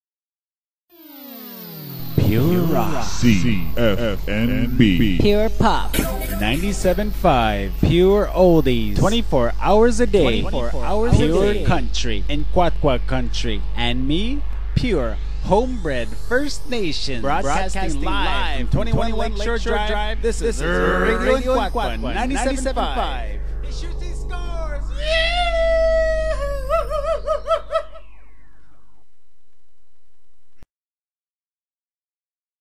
Pure Rock Commercial N'Quatqua 97 5 FM
a Commercial/Station ID I created for N'Quatqua 97.5 FM in D'Arcy, BC